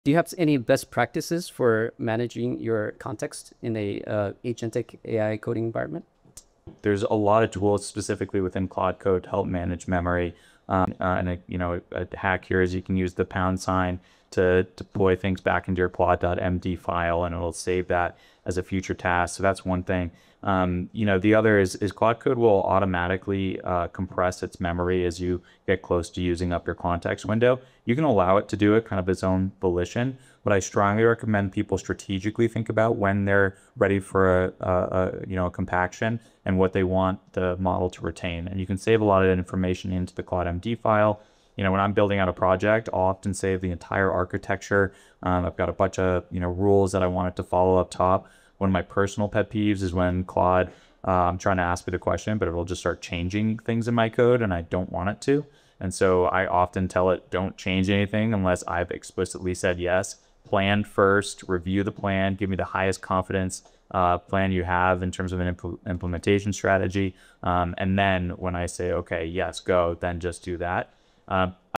Claude Code Context Management: Pro Tips from Anthropic
Eli Chen asks Paxton Maeder-York from Anthropic about managing context in agentic AI coding environments. Key tips: Use # to save tasks to plot.md, strategically time memory compression instead of letting Claude do it automatically, and always make Claude plan first before changing code. "Don't change anything unless I've explicitly said yes - plan first, review the plan, then execute."